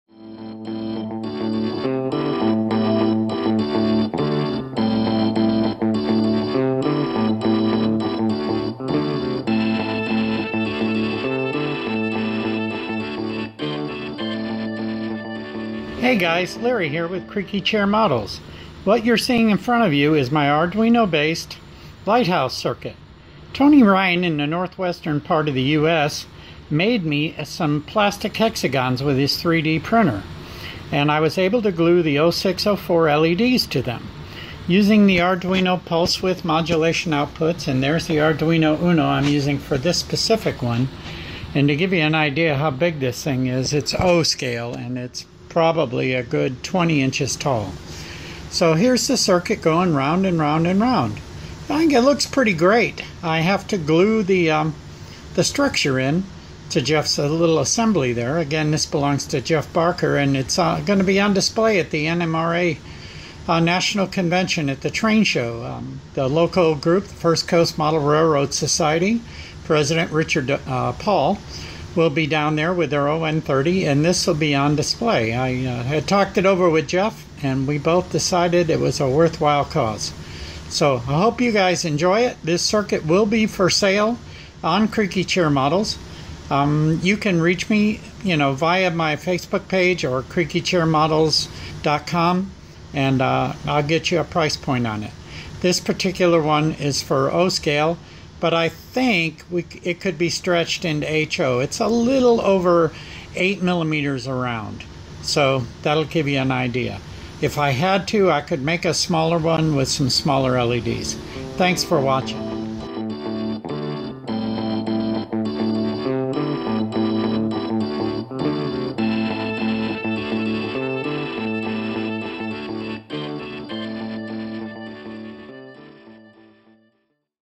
The Lighthouse circuit
This is a video of my Arduino based lighthouse circuit that simulates a rotating beacon using 6 LED's.